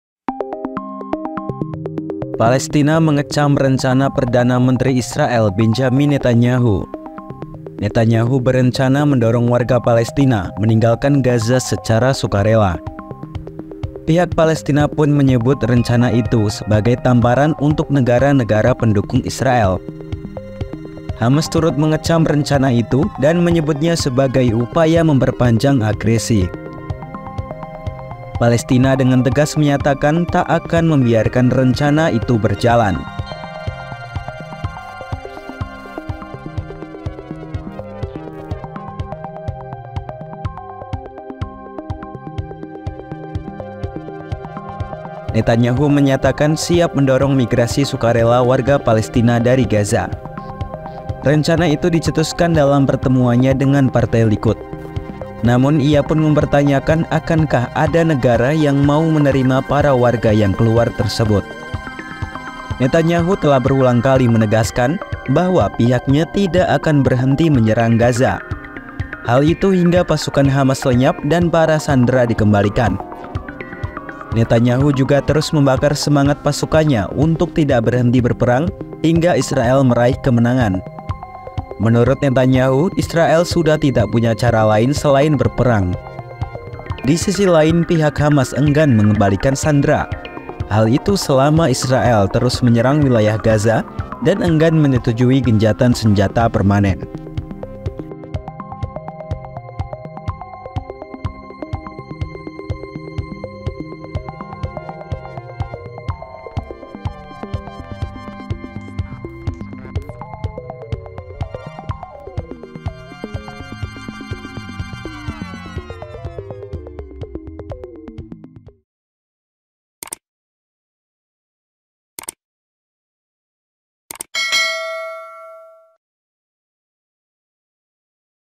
Palestina Kecam Rencana Netanyahu Usir Warga Keluar dari Gaza
Perdana Menteri Israel Benjamin Netanyahu disebut berencana mendorong warga Palestina untuk keluar dari Gaza secara sukarela.

Mengetahui hal itu, pihak Palestina pun mengecam Netanyahu atas rencananya tersebut.

Pihak Palestina menilai rencana itu sebagai tamparan untuk negara-negara yang mendukung Israel dalam perang di Jalur Gaza.

Sementara kelompok Hamas turut mengecam rencanan Netanyahu dengan menyebutnya sebagai upaya memperpanjang agresi.

Netanyahu pun menegaskan bahwa Israel tetap akan menyerang Gaza hingga Hamas dilenyapkan dan para sandera dikembalikan.

Penulis Naskah: Muhammad Dava Arrifa
Narator: Muhammad Dava Arrifa
Video Editor: Tri Febrianto Gunawan
Produser: Adisty Safitri